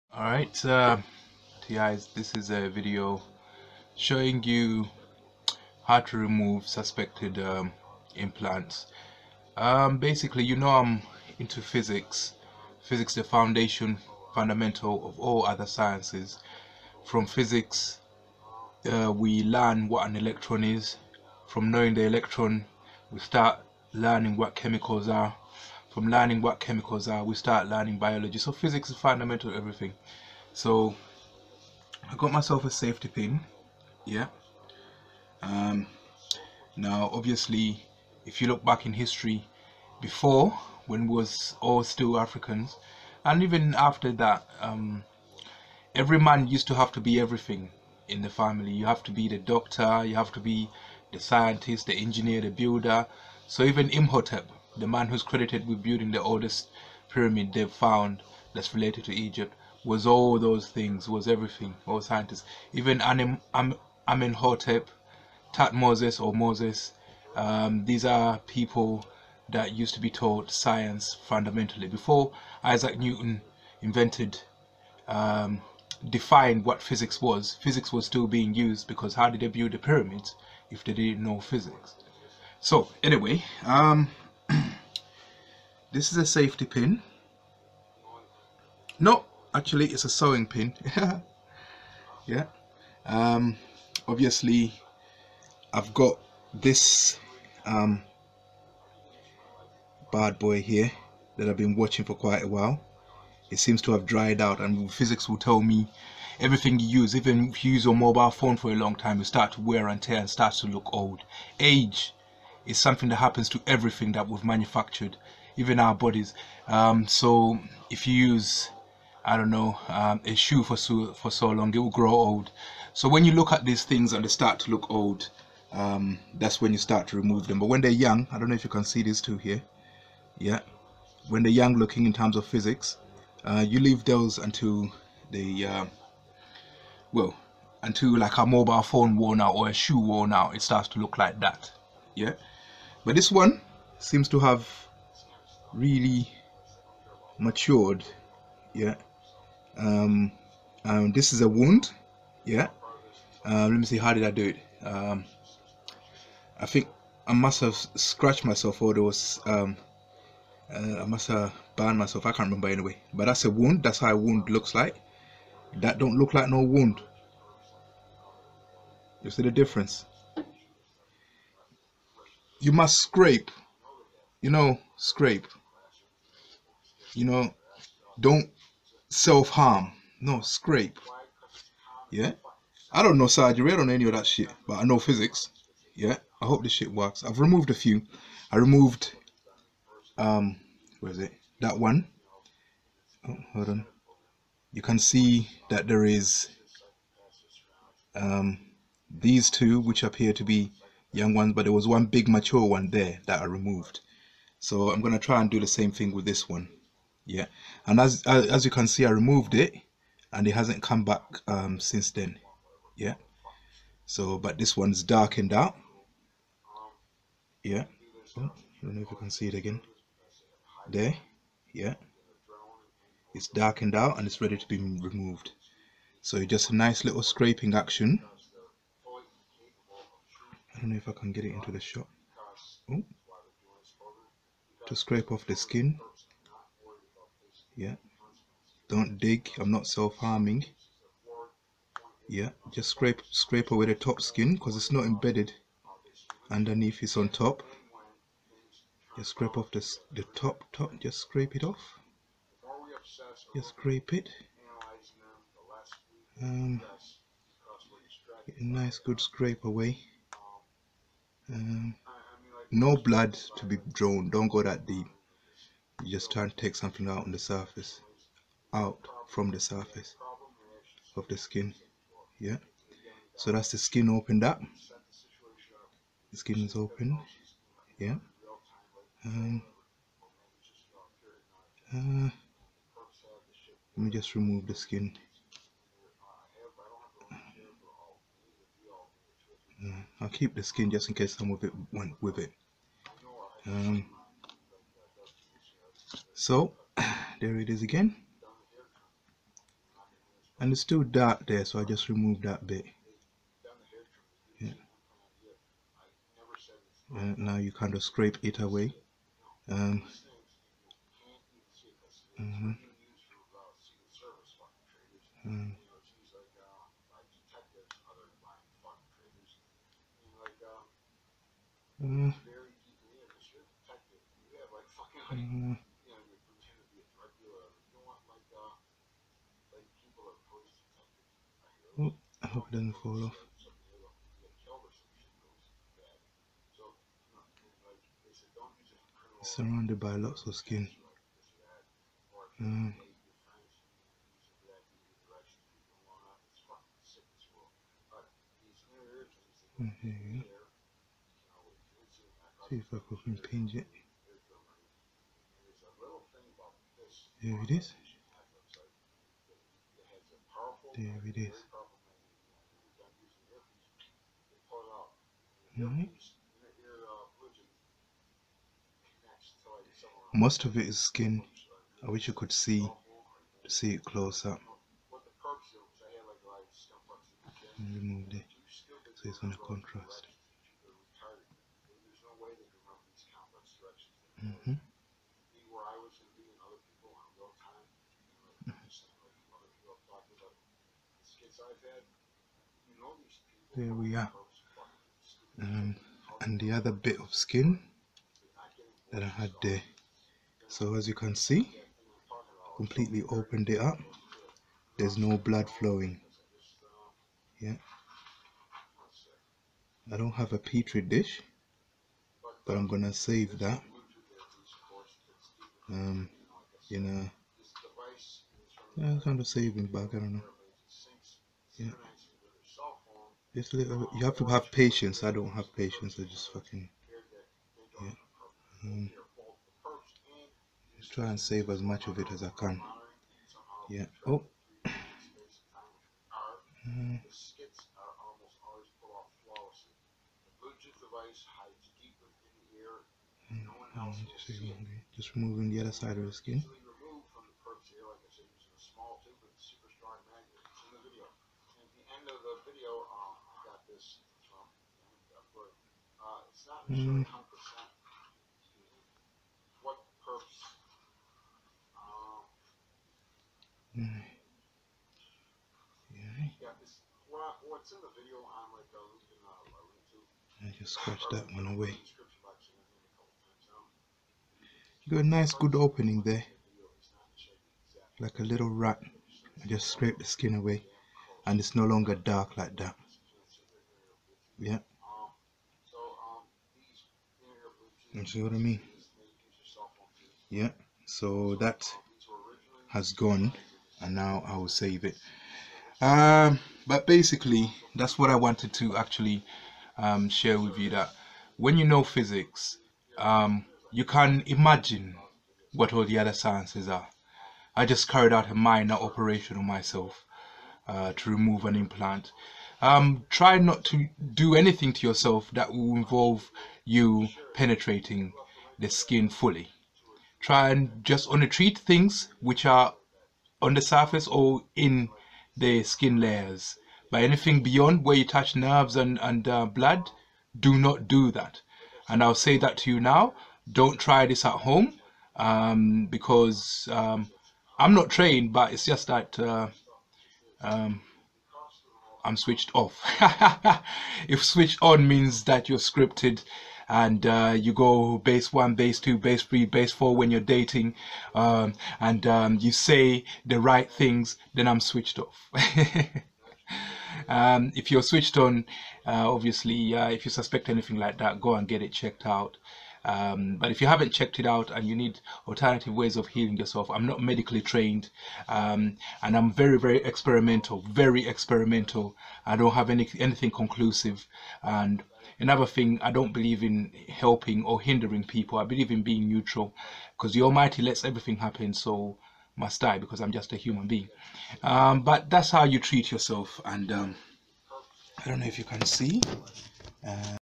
Targeted Individual - how to remove skin-deep anomalies from your body - recorded demonstration
we all know this in our cultures yet we have never considered how much we have to learn in order to know how to do it. biology, dermatology etc spring to mind but if i consider it in terms of physics it makes more sense (to me).

"do not self-harm"